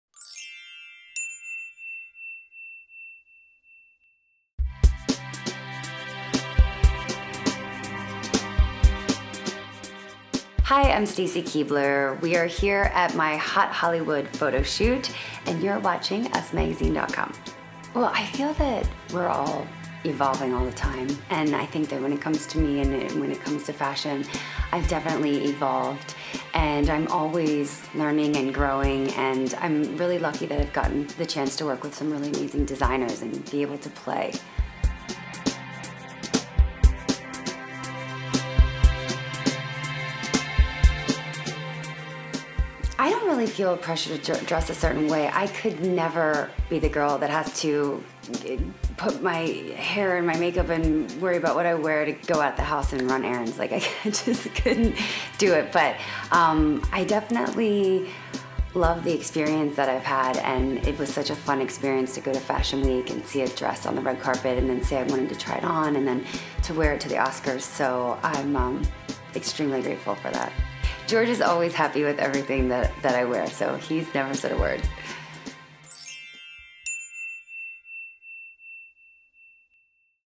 Stacy Keibler: George Clooney Is Happy With Everything I Wea
"He doesn't care whether I'm dressed up or down," the Us Hot Hollywood honoree says of her boyfriend